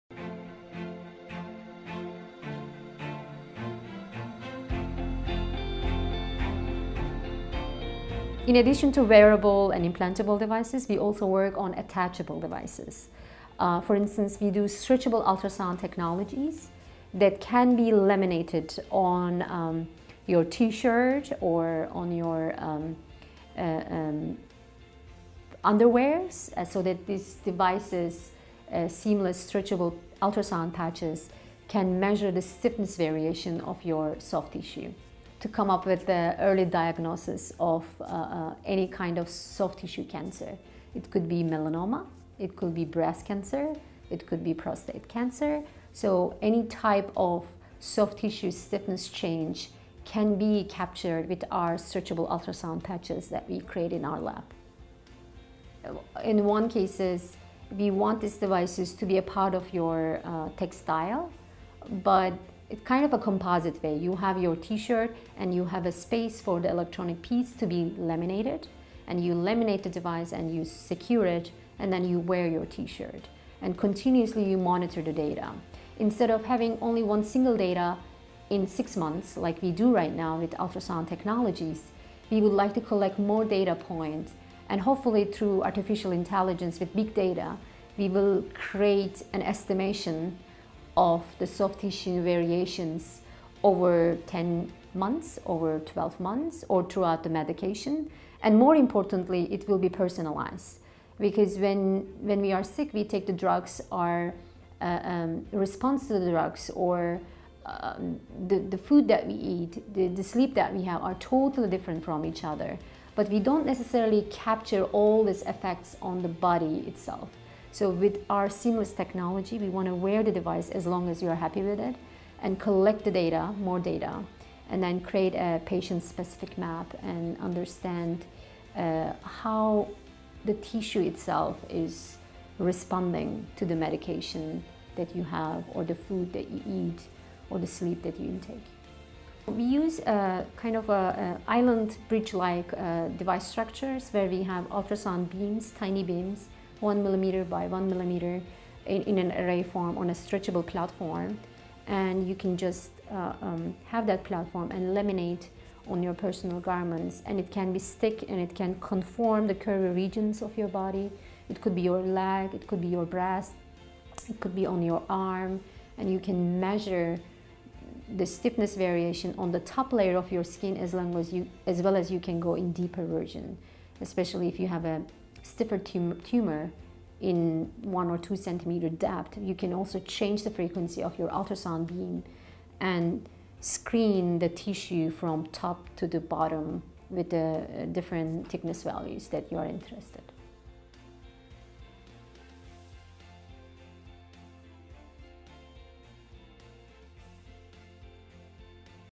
Designing Medical Devices to Bend with the Body - Attachable Devices - 3 of 6
Canan Dagdeviren, LG Career Development Professor of Media Arts & Sciences, MIT Media Lab - See Canan's full playlist